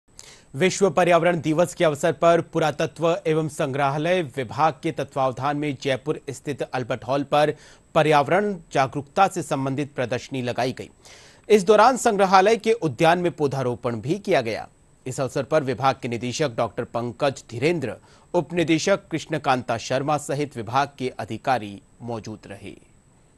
विश्व पर्यावरण दिवस के अवसर पर अल्बर्ट हॉल पर पर्यावरण जागरूकता से संबंधित लगाई गई प्रदर्शनी
Jaipur : विश्व पर्यावरण दिवस के अवसर पर अल्बर्ट हॉल पर पर्यावरण जागरूकता से संबंधित लगाई गई प्रदर्शनी
विश्व पर्यावरण दिवस के अवसर पर पुरातत्व एवं संग्रहालय विभाग के तत्वावधान में जयपुर स्थित अल्बर्ट हॉल पर पर्यावरण जागरूकता से संबंधित प्रदर्शनी लगाई गई
इस दौरान संग्रहालय के उद्यान में पौधारोपण भी किया गया
इस अवसपर विभाग के निदेशक डॉ पंकज धीरेंद्र, उप निदेशक कृष्णकांता शर्मा सहित विभाग के अधिकारी रहे मौजूद